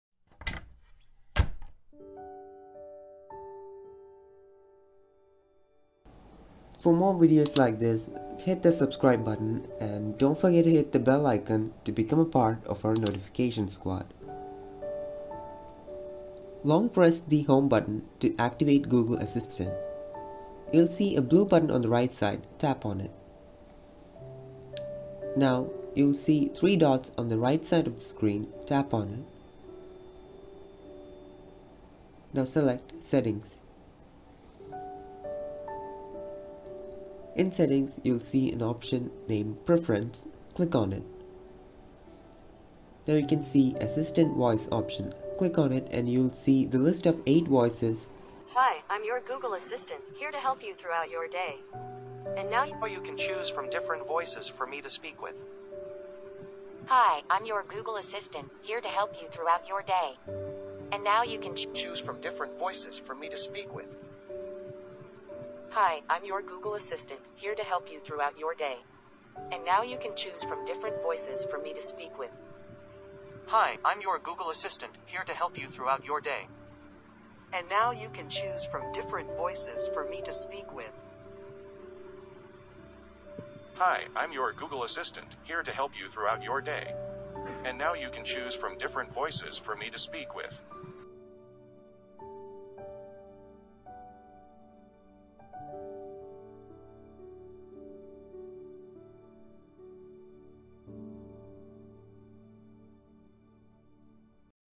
Change Google Assistant's Voice | 8 New Voices
How to change google assistant's voice. There are 8 new voices added in the latest update.